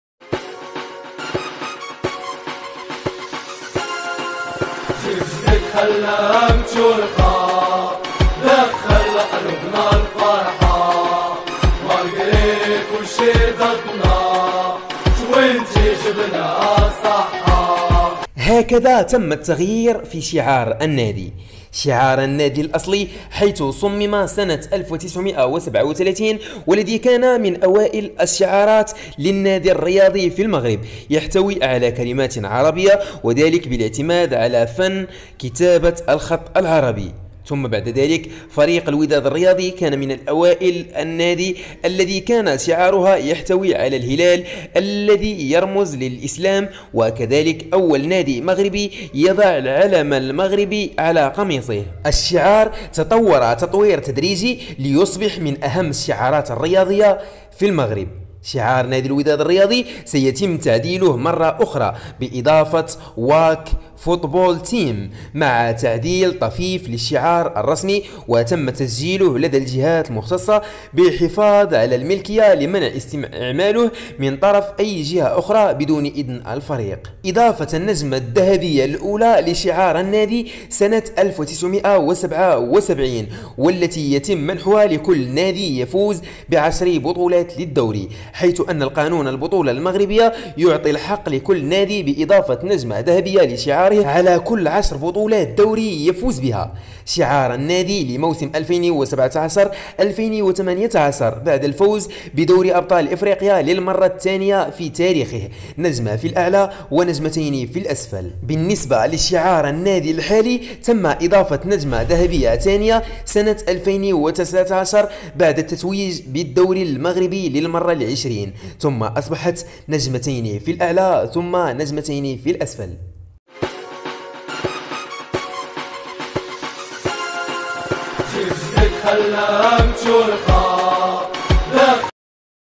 ثاريخ الوداد🔥 Ep4 - هاكذا تطور شعار الوداد الرياضي مند 84 عاما 🔥 لا يفوتك
الوداد اللاعبون

ميركاتو الوداد

مباريات الوداد

ألقاب الوداد

الوداد اللاعبون